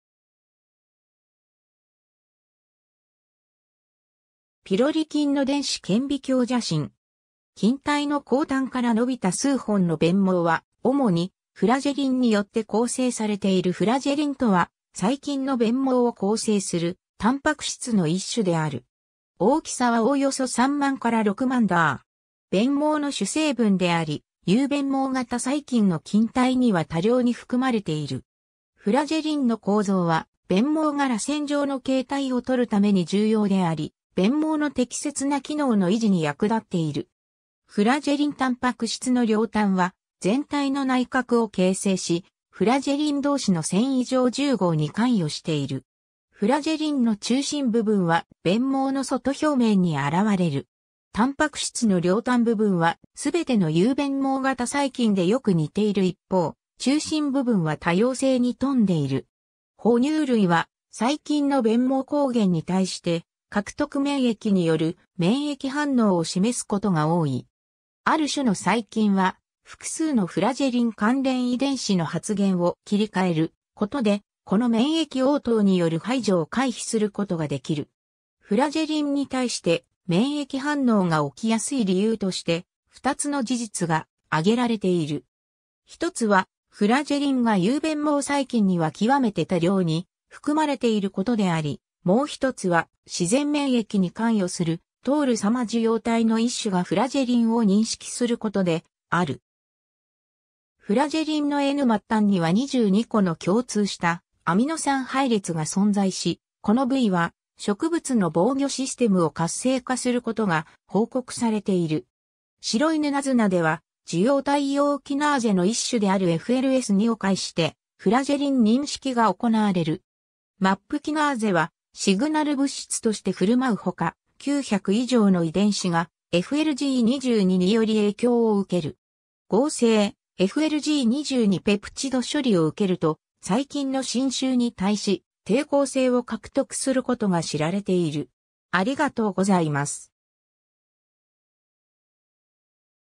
フラジェリン
ピロリ菌の電子顕微鏡写真。
菌体の後端から伸びた数本の鞭毛は、主にフラジェリンによって構成されている フラジェリン（英：flagellin）とは、細菌の鞭毛を構成するタンパク質の1種である。
大きさはおおよそ3万～6万Da。
鞭毛の主成分であり、有鞭毛型細菌の菌体には多量に含まれている。
 フラジェリンの構造は鞭毛がらせん状の形態をとるために重要であり、鞭毛の適切な機能の維持に役立っている。
 フラジェリンタンパク質の両端（N末端およびC末端）は全体の内核を形成し、フラジェリン同士の繊維状重合に関与している。
フラジェリンの中心部分は鞭毛の外表面に現れる。
タンパク質の両端部分は全ての有鞭毛型細菌でよく似ている一方、中心部分は多様性に富んでいる。
 哺乳類は細菌の鞭毛抗原に対して、獲得免疫（抗原提示およびT細胞が関与する）による免疫反応を示すことが多い。
ある種の細菌は複数のフラジェリン関連遺伝子の発現を切り替えることで、この免疫応答による排除を回避することができる。
 フラジェリンに対して免疫反応が起きやすい理由として、2つの事実が挙げられている。
1つはフラジェリンが有鞭毛細菌には極めて多量に含まれていることであり、もう1つは自然免疫に関与するToll様受容体の1種（TLR5）がフラジェリンを認識することである。
 フラジェリンのN末端には22個の共通したアミノ酸配列（flg22）が存在し、この部位は植物の防御システムを活性化することが報告されている。
シロイヌナズナでは、受容体様キナーゼの1種であるFLS2（flagellin-sensitive-2）を介してフラジェリン認識が行われる。
MAPキナーゼはシグナル物質として振舞うほか、900以上の遺伝子がflg22により影響を受ける。
合成flg22ペプチド処理を受けると、細菌の侵襲に対し抵抗性を獲得することが知られている。